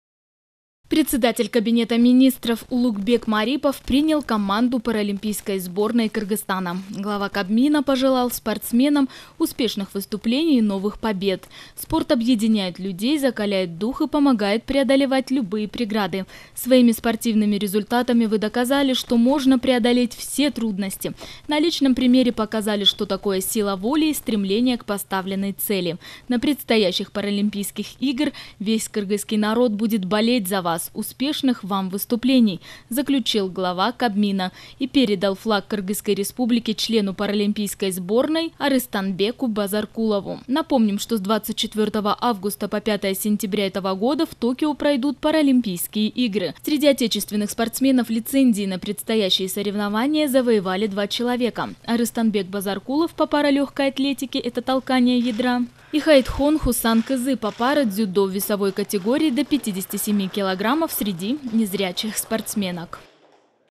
Улукбек Марипов: Вся страна верит в победу отечественных спортсменов на Паралимпийских играх в Токио
Председатель Кабинета Министров Улукбек Марипов принял команду Паралимпийской сборной Кыргызской Республики. Глава Кабмина пожелал спортсменам успешных выступлений и новых побед.